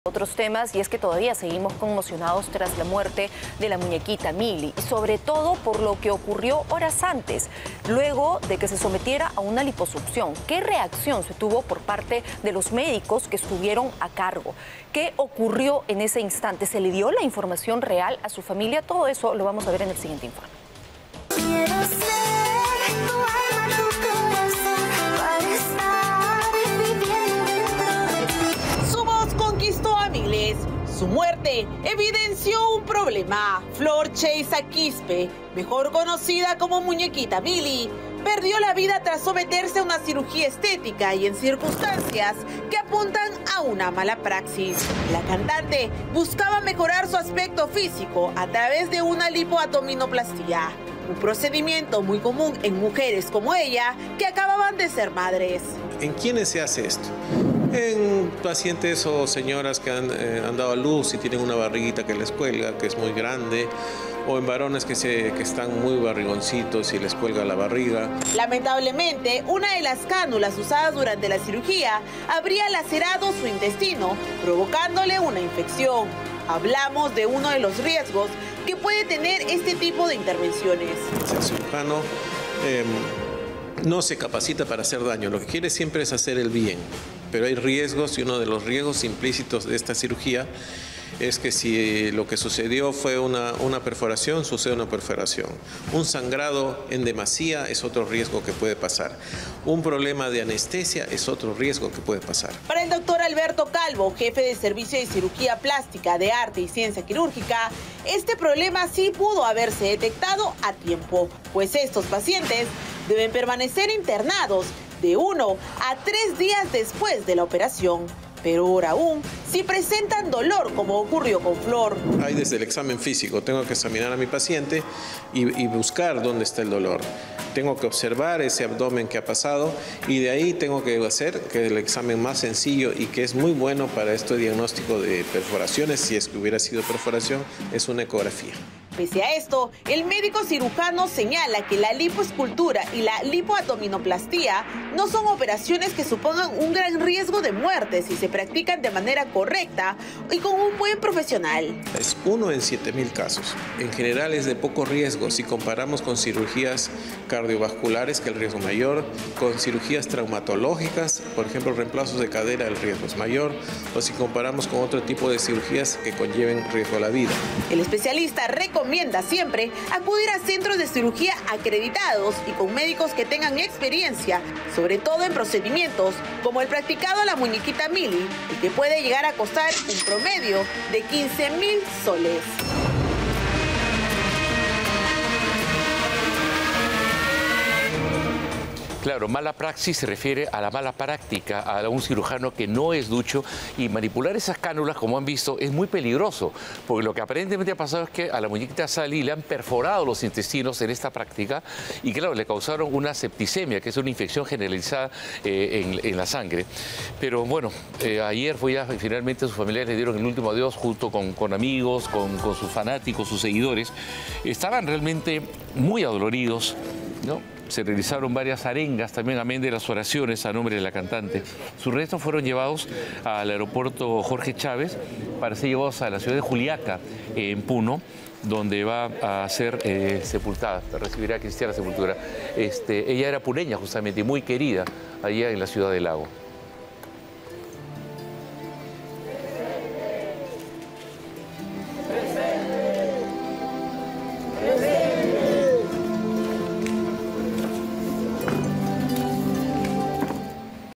¿ Por qué murió la "Muñequita Milly " ? Dr. Alberto Calvo, cirujano plástico. Willax TV
Dr. Alberto Calvo,. cirujano plástico